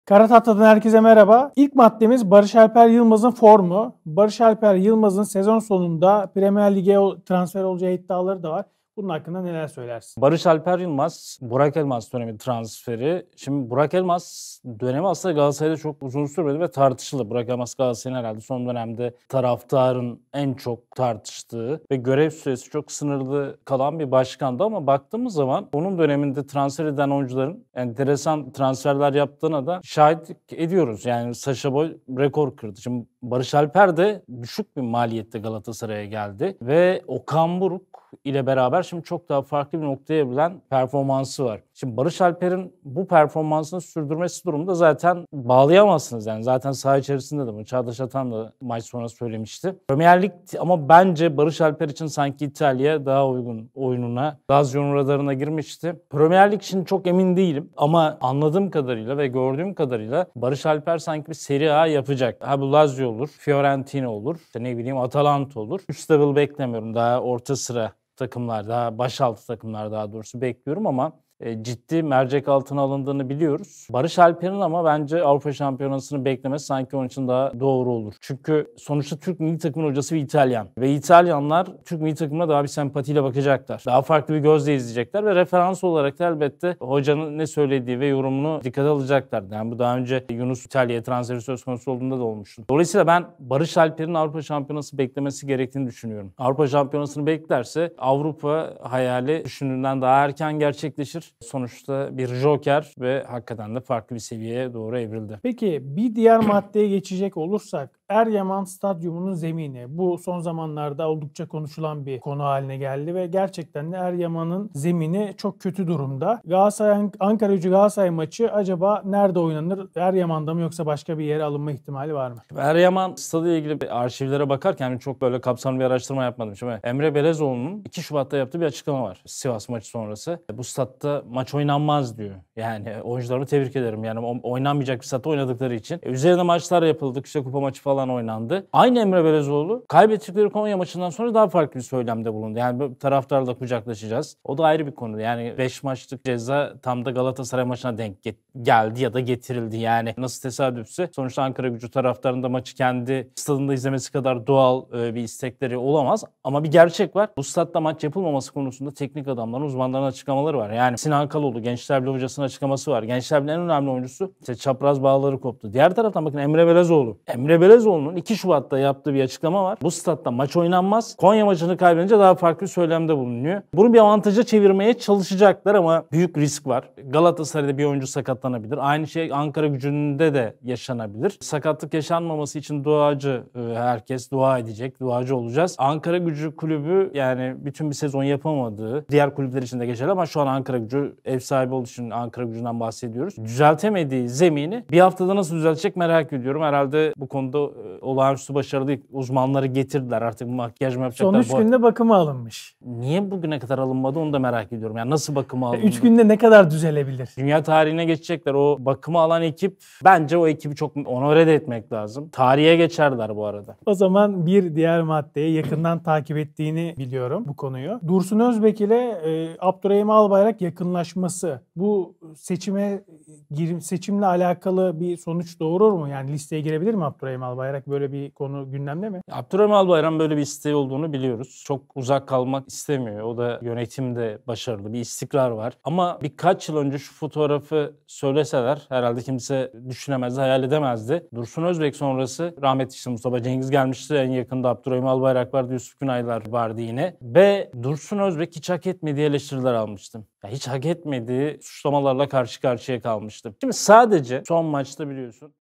Abdurrahim Albayrak Galatasaray’ın yeni yönetiminde olacak mı? - “Barış Alper, Beklemeli”
Nevzat Dindar, 343 Digital’de Galatasaray gündemini belirliyor.